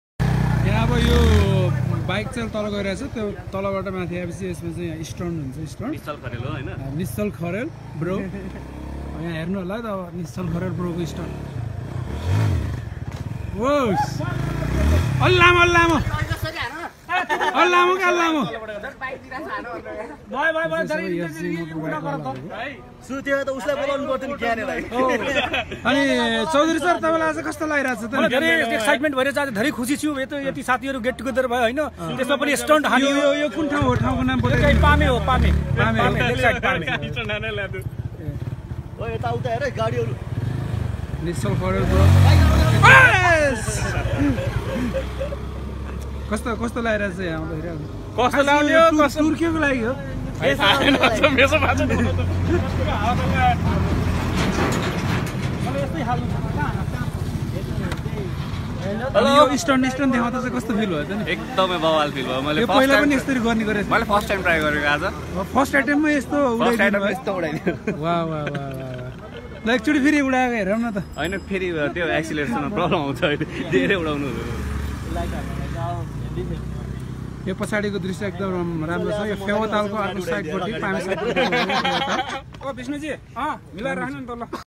Bike Ride|FZ25| Friends tour to Paame Pokhara|mypromiseland
FZ25 bike riders in Pokhara, Nepal
For Trending: @mypromisland